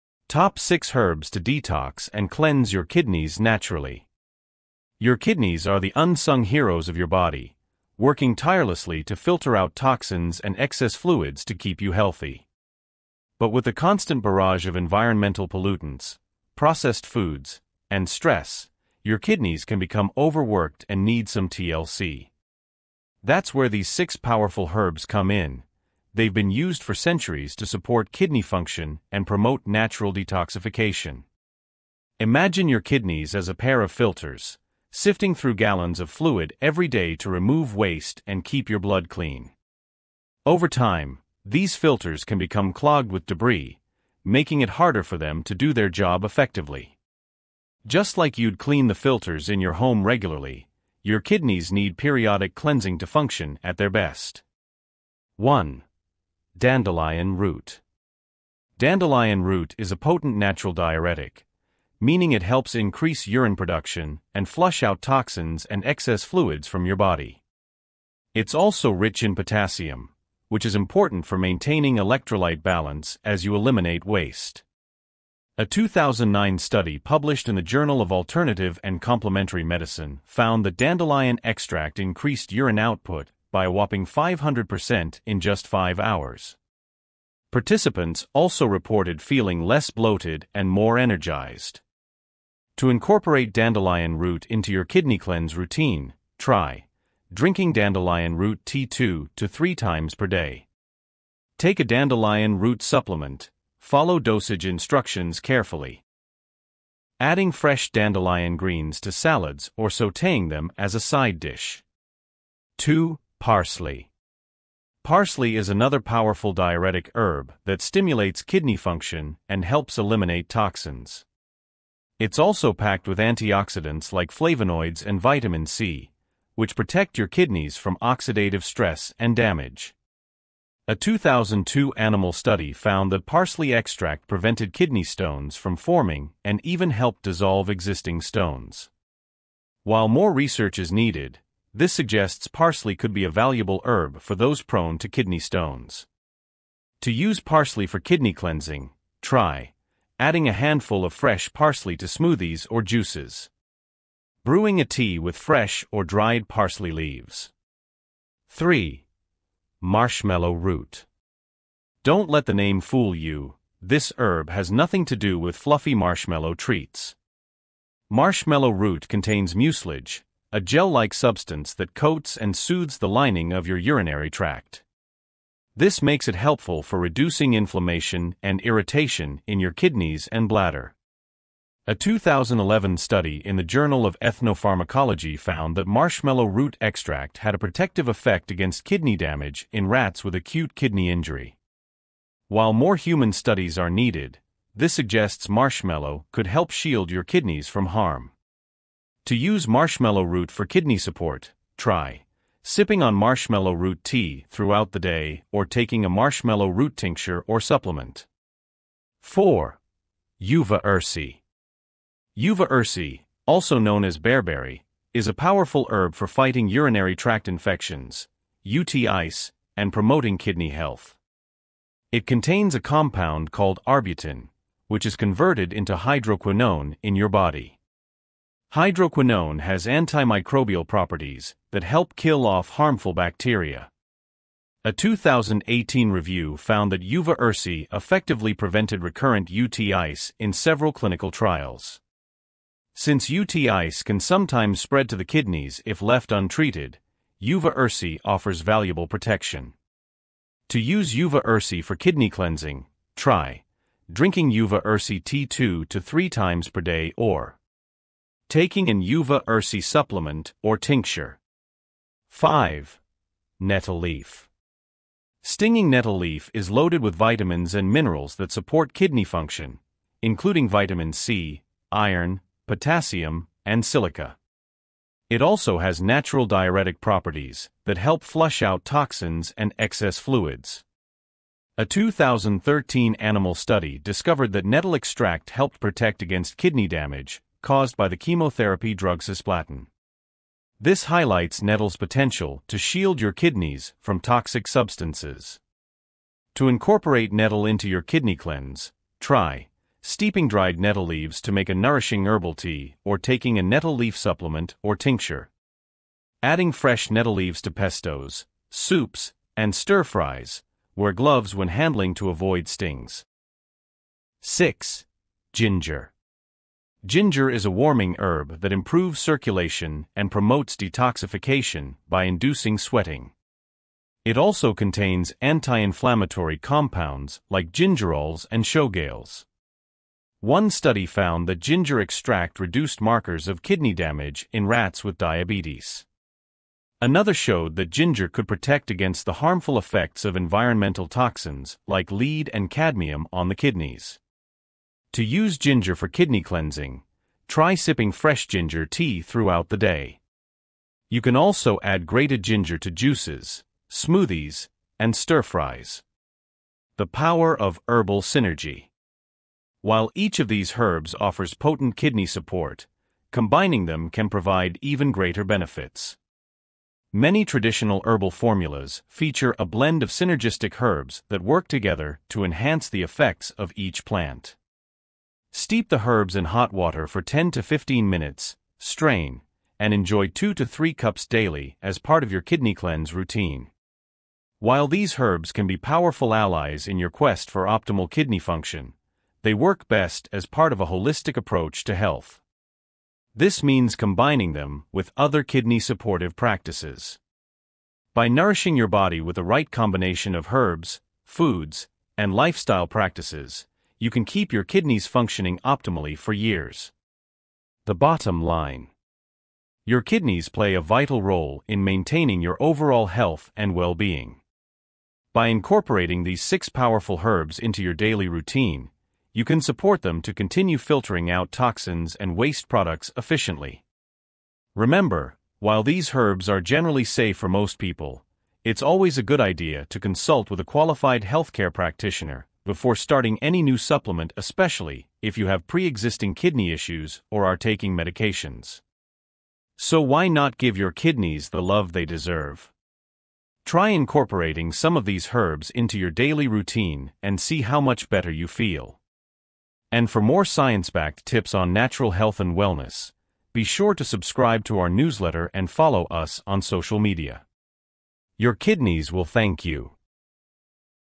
Top 6 Herbs to DETOX and CLEANSE Your KIDNEYS Naturally
Discover the top 6 herbs that can help DETOX and CLEANSE your kidneys naturally!

Are you tired of feeling sluggish and run down? Do you want to improve your overall health and wellbeing?

Your kidneys play a crucial role in filtering out toxins and waste from your body, but sometimes they need a little extra support.

In this video, we'll explore the 6 most effective herbs to help boost your kidney function, reduce inflammation, and promote a healthy detox.

Take the first step towards a healthier you! Watch now and start your journey to naturally cleanse and detox your kidneys!

⏰Timestamps:
00:00 - Intro
00:55 - Dandelion Root
01:52 - Parsely
02:38 - Marshmallow Root
03:29 - Uva Versi
04:22 - Nettle Leaf
05:14 - Ginger
05:58 - The Power of Herbal synergy